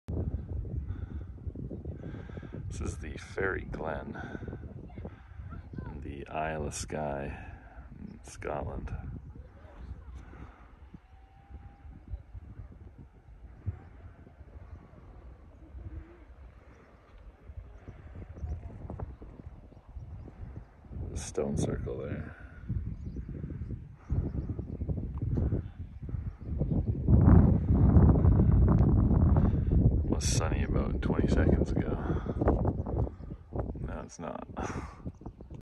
The Fairy Glen in the Isle of Skye
This video shows the Fairy Glen on the Isle of Skye. Located above the village of Uig (see my previous vido of Uig!) in the Trotternish penninsula, this incredible landscape was apparently shaped by a landslip. The green grass and lochans (ponds) gives the place an almost otherworldly feel.

The Fairy Glen is definitely a worthwhile stop during any tour of Skye- as if anyone needed an additional reason to visit!